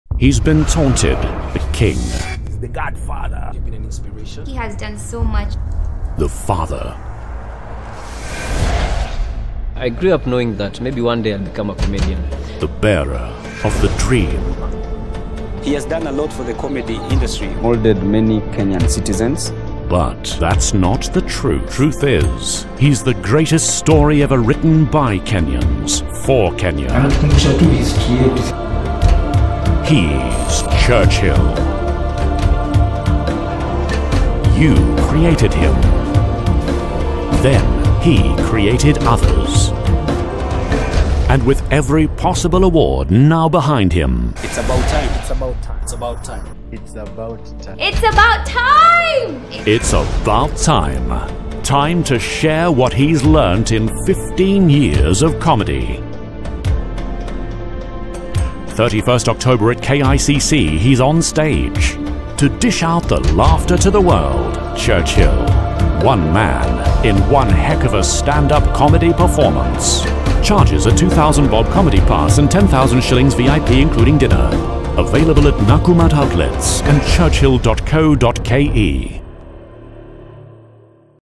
Night With Churchill TVC - 2015
On this tv commercial for 'A night with Churchill' produced by J Blessing, we provided the close up aerial shots and the smooth gimbal walking shots.